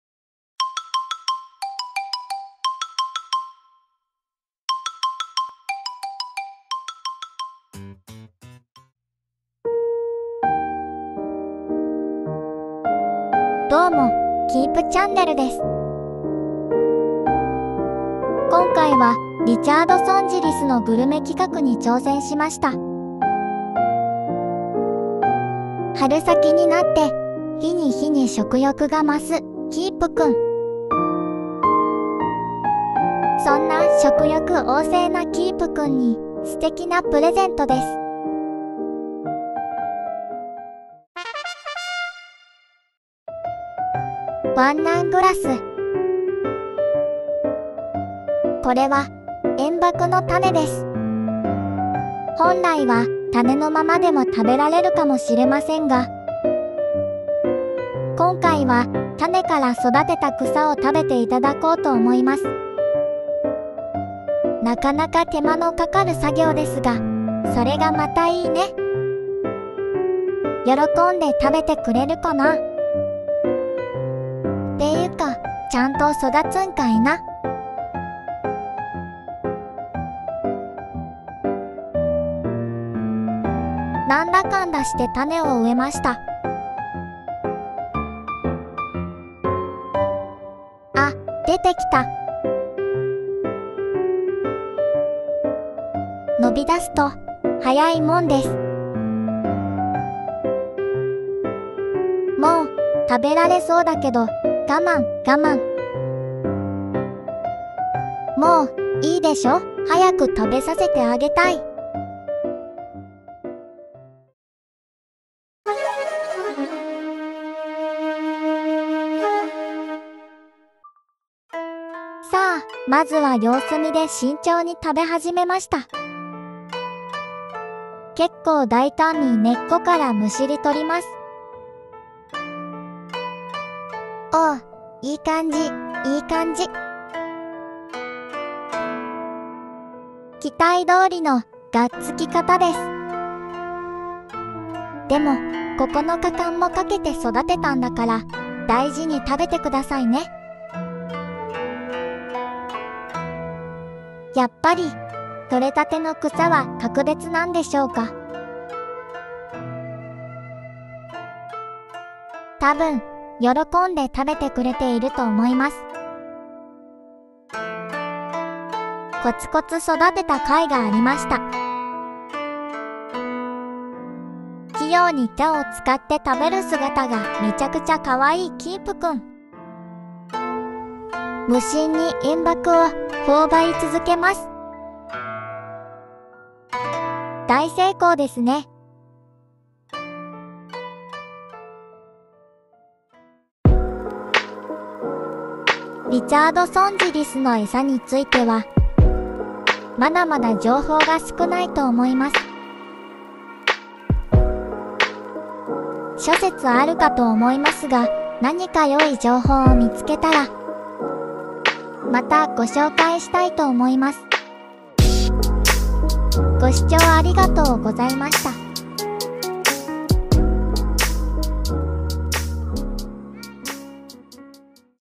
初めての生えん麦食べ放題【リチャードソンジリス】
食欲旺盛のKEEP君(リチャードソンジリス)。
いつもとはひと味違う食事を準備してみました。
果たして喜んでいただけるでしょうか…